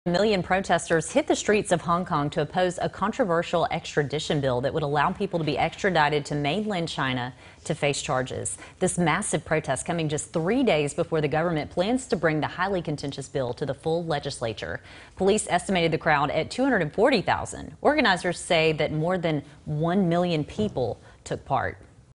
New extradition law draws huge protests in Hong Kong
More than a million protesters hit the streets of Hong Kong to oppose a controversial extradition bill that would allow people to be extradited to mainland China to face charges.